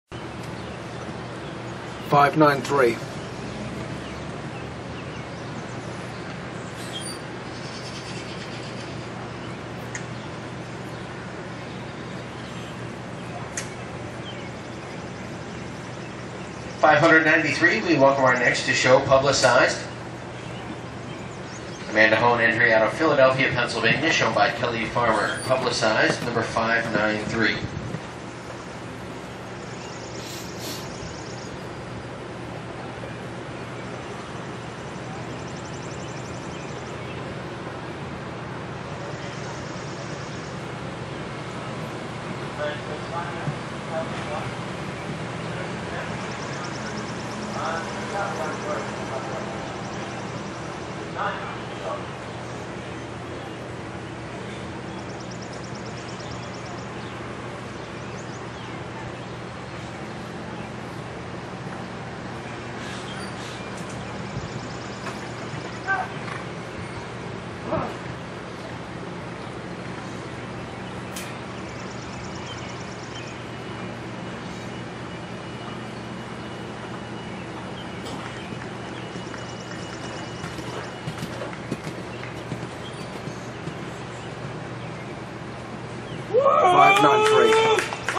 Video of PUBLICIZED ridden by KELLEY FARMER from ShowNet!
PUBLICIZED ridden by KELLEY FARMER at Kentucky Spring I 2016.  This round is from class 12, FIRST YEAR GREEN HUNTER and finished 1 of 17.